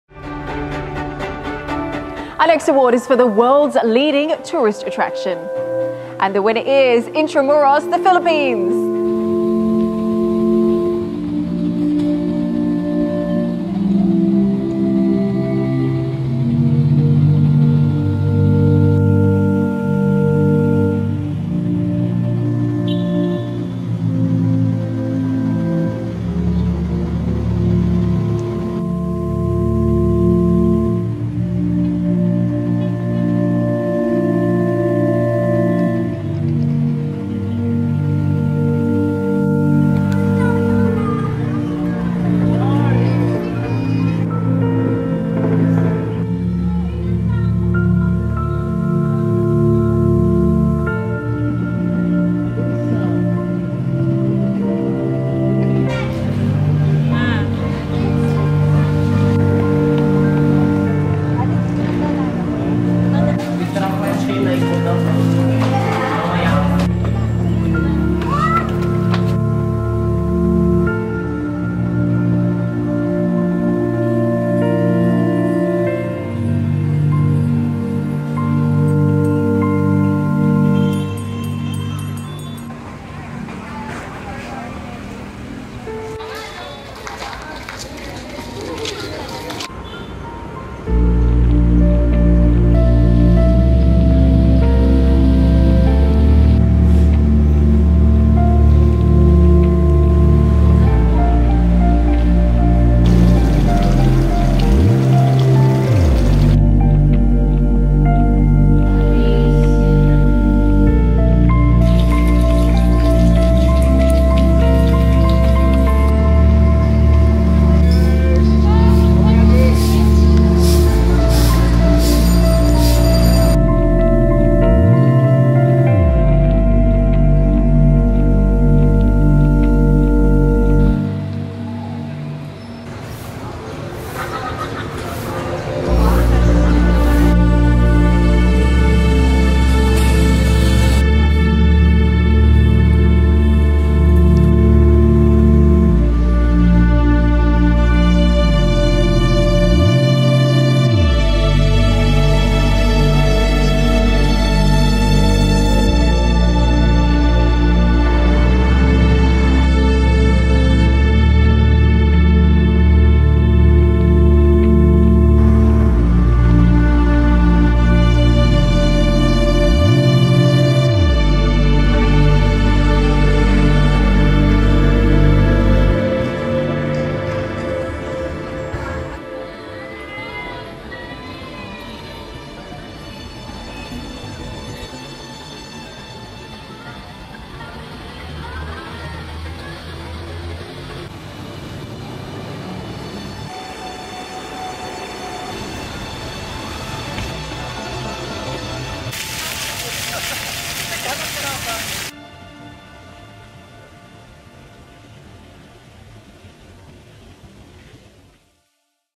Winner 2020 World's Leading Tourist Attraction is Intramuros World Travel Awards
Intramuros, Manila in the Philippines is this year's winner for the World's Leading Tourist Attraction. This is awarded by the World Travel Awards. Video footages done before the pandemic.